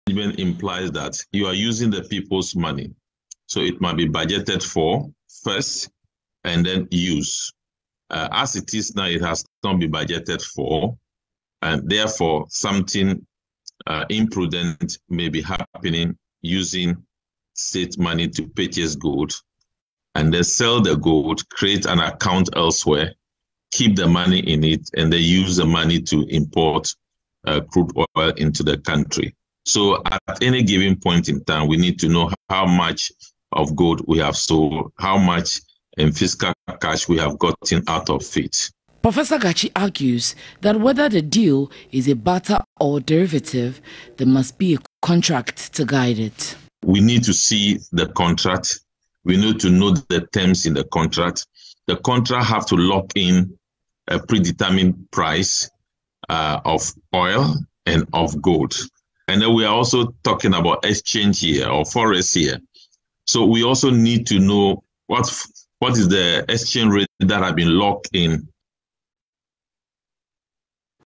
Professor Gatsi describes gold for oil transaction as illegal.
Dean of the University of Cape Coast Business School, Professor Gatsi has described transactions under government’s ‘gold for oil’ deal as illegal as it does not have any parliamentary approval.